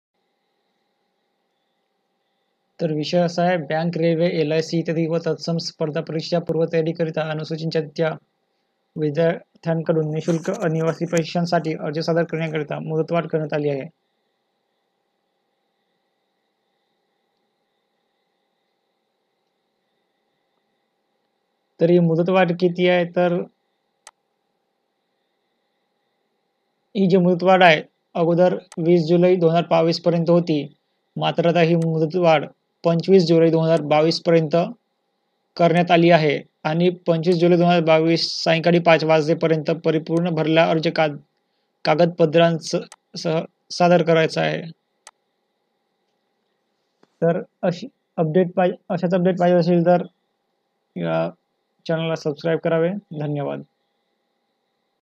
barti -Extension of time for submission for Bank, Railway, LIC and similar competitive examinations
Extension of time for submission of applications for free residential training for preparation of Bank, Railway, LIC and similar competitive examinations